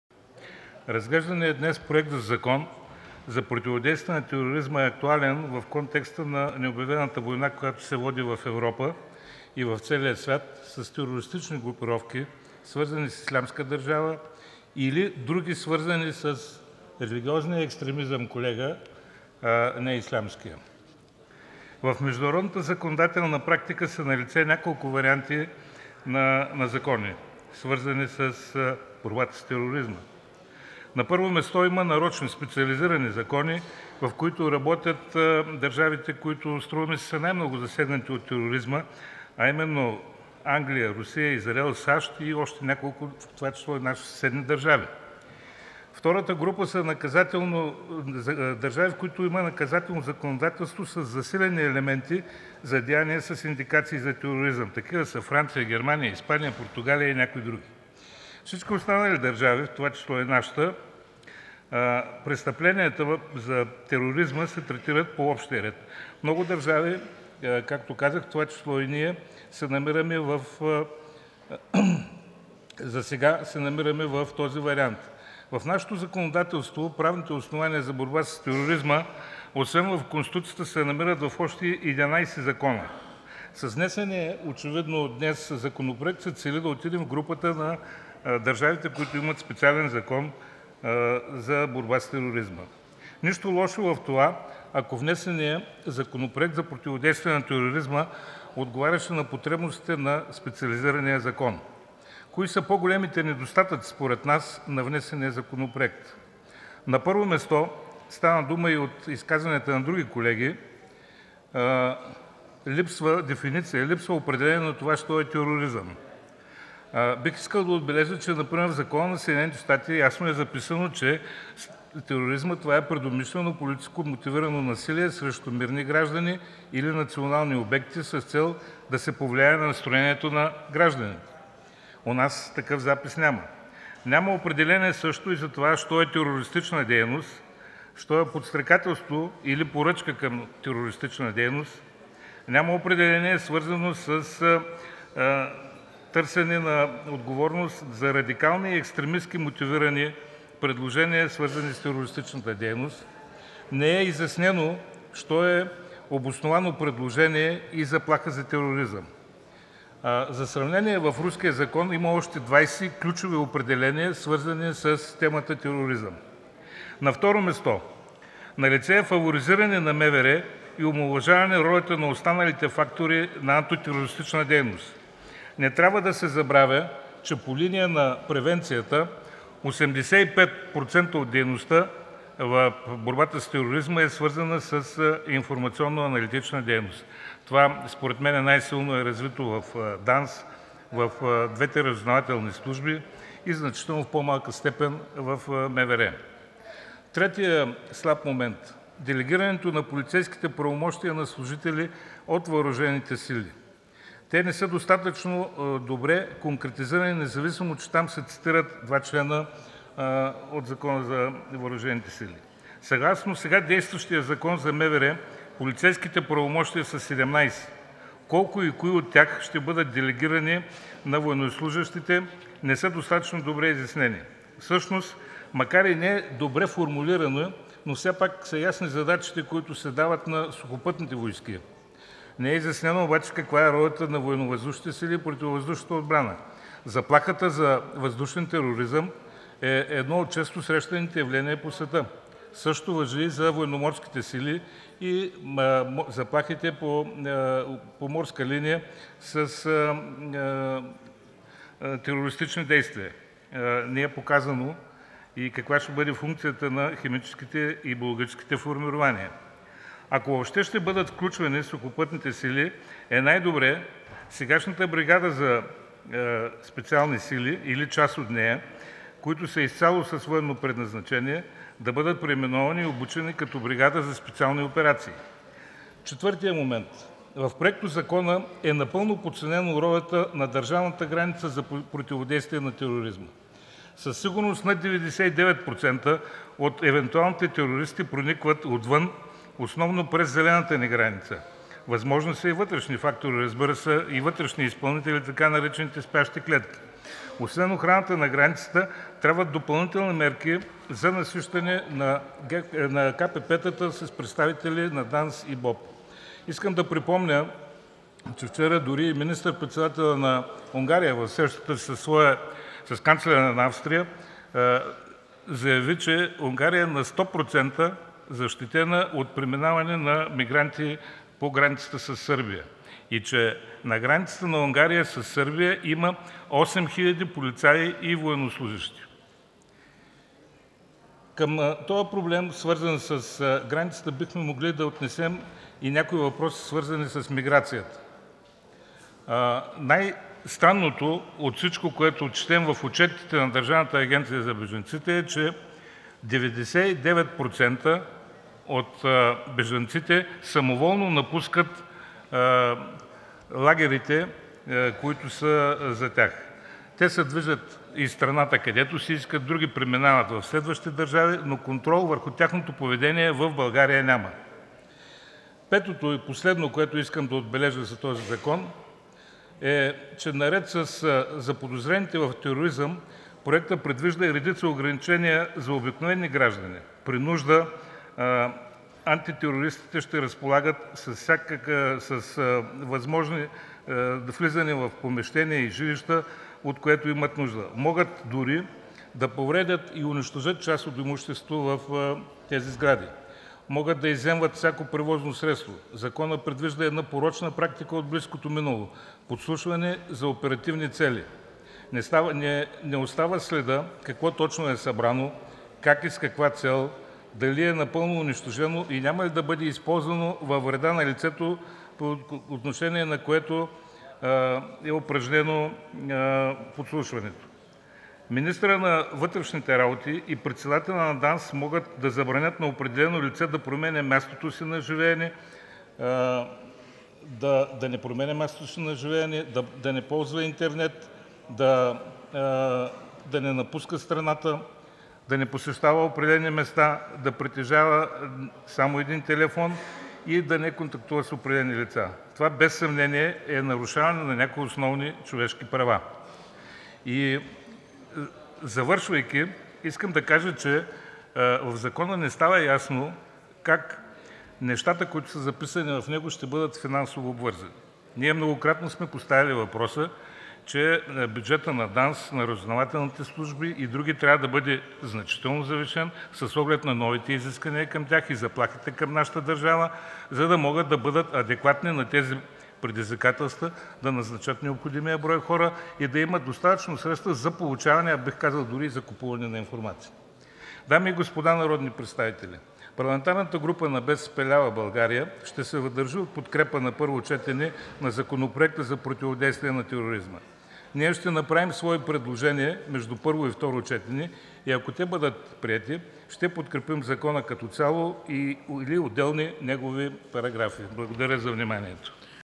Димитър Дъбов: Законопроектът за тероризма не отговаря на потребностите на специализиран закон
„С внесения законопроект за тероризма сме на път да отидем в групата на специализираните закони. Нищо лошо, но ако той отговаряше на потребностите на специализиран закон”.